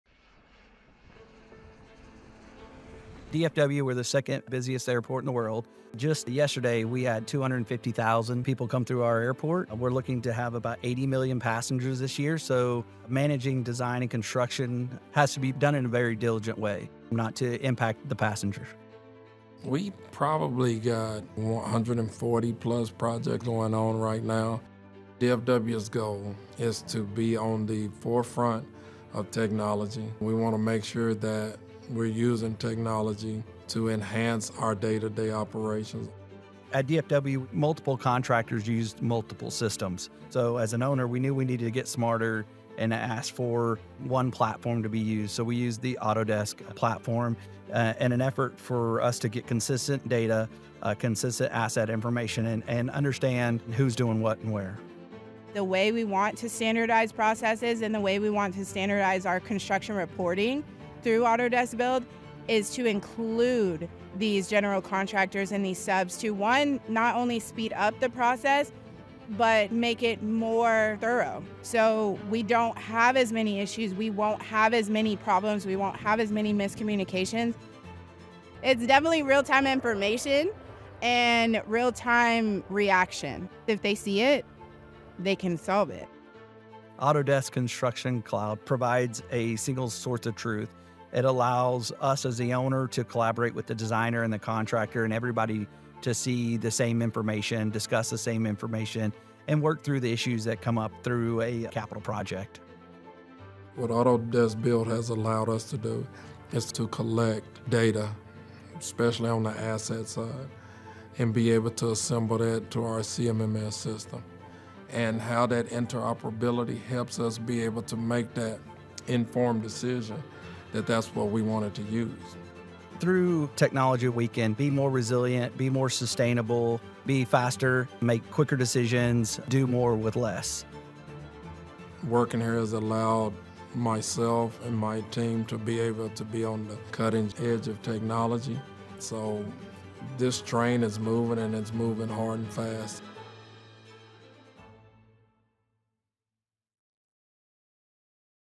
Revolutionizing Airport Construction: DFW's Success Story
Join us in this exclusive behind-the-scenes look at Dallas Fort Worth International Airport (DFW), the world's second busiest airport.

With a staggering 80 million passengers expected this year, DFW is embracing cutting-edge technology to manage its 140+ construction projects and ensure a seamless passenger experience.
Learn how Autodesk Construction Cloud's unified platform is transforming the way DFW manages design, construction, and asset data, leading to real-time collaboration and faster, more informed decision-making. Discover how DFW Airport is staying ahead of the game in this tech-driven era.